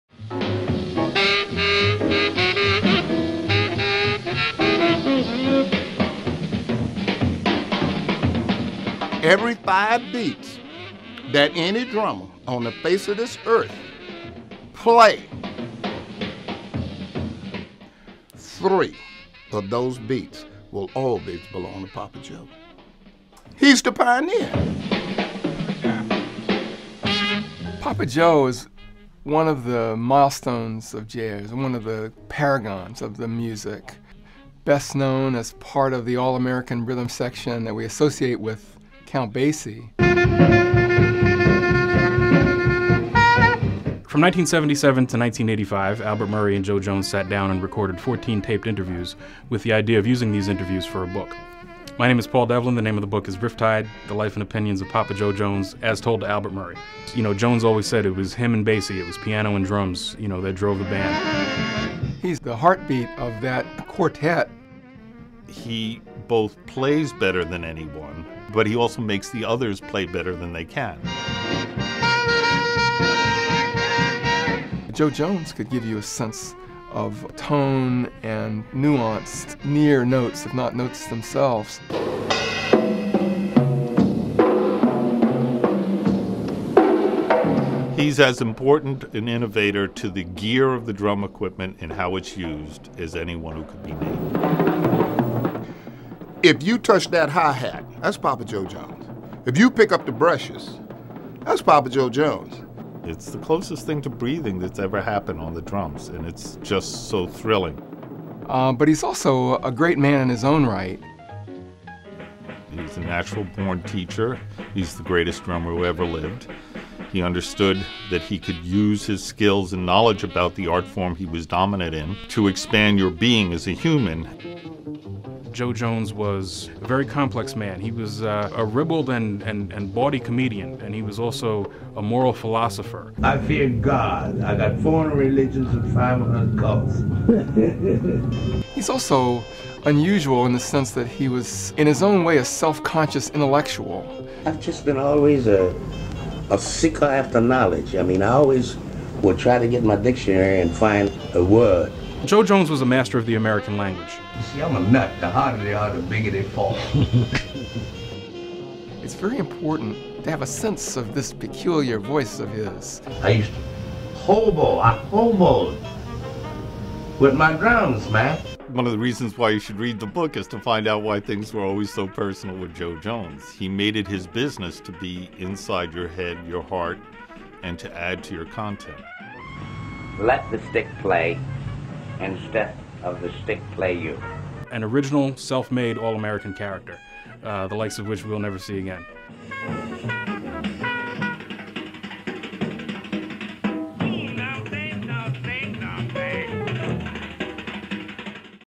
RIFFTIDE: The Life and Opinions of Papa Jo Jones (book trailer)
"Jo Jones, an elegant, swinging dude, always had a style of his own. When he was with us, you could hear him, feel him—everything was right there."—Count Basie

"This is my last hoo-rah." So begins the autobiography of Jonathan David Samuel Jones—or as the world better knows him, Papa Jo Jones. Playing with Count Basie and his orchestra when they exploded out of Kansas City in 1936 and took the world by storm, Jones went on to inspire generations of jazz drummers, but until now few have had access to his own remarkable story.

A true American original and jazz luminary, Papa Jo Jones bedazzled and intrigued many with his outrageous, volatile personality and his innovative drumming—and nowhere does his fierce intellect and humor shine more marvelously than in his life's telling. With a fascinating introduction and annotations by Paul Devlin and an afterword by Phil Schaap, jazz historian and longtime friend of Jones, RIFFTIDE reveals a man at the forefront of both a whole new form of music and a country in the midst of incredible turmoil and opportunity. As Jones himself puts it: "Listen man, I've had a hell of a time . . ."

"I first met Jo Jones at the RKO Theater in Boston when I was a teenager in the early 1940s and we were friends until he passed away. He was my first influence and my major influence. He was 'Papa' Jo to me before they gave him that title. He was like a father to me. For drummers of my generation, Jo was the president of the drums just like Lester Young was president of the tenor saxophone. Jo loved to talk, and when he spoke it was almost as if he was playing the drums: you'd give him your undivided attention. Rifftide conveys a fine sense of his voice and the larger than life dimensions of his personality."—Roy Haynes